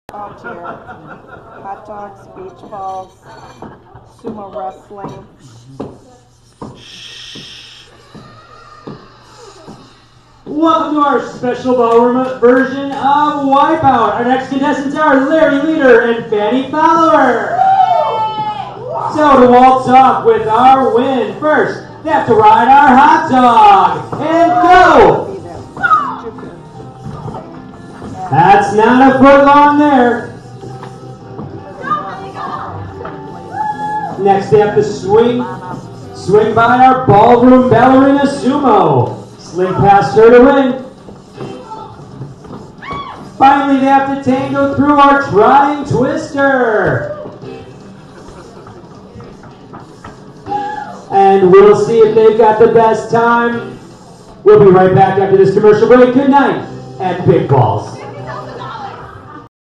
Festival Premiere Party Skit 1 - Fun and Games at Davian's
The first party of this year's festival will be at Davian's where there will sumo suits, a mechanical cheeseburger, an inflatable twister, dancing, food and drinks!!!  Can you tell that from this skit?

If you would like more information on being a sponsor please see you instructor, Ms. Lori, or Ms. Erin for more details.